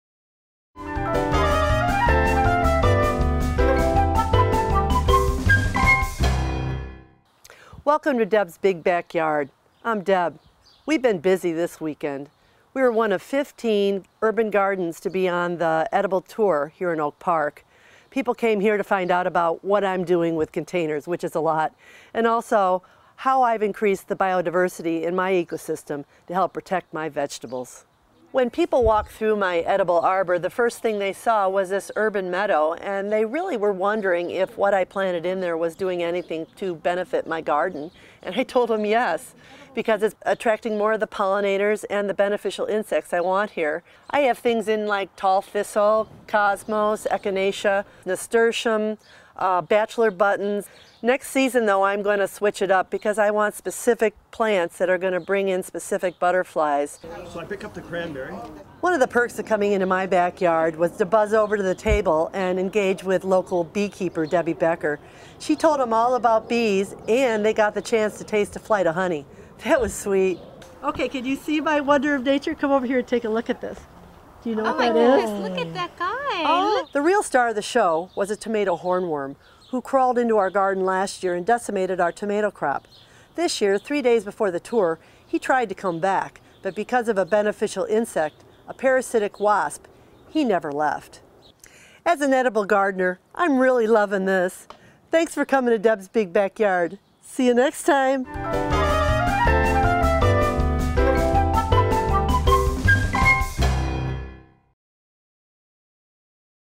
Deb's Big Backyard: Edible Garden Tour
Deb Quantock McCarey highlights her urban meadow and Tomato Hornworm meets Parasitic Wasp from the Sugar Beet Coop Edible Garden Tour on July 27, 2013.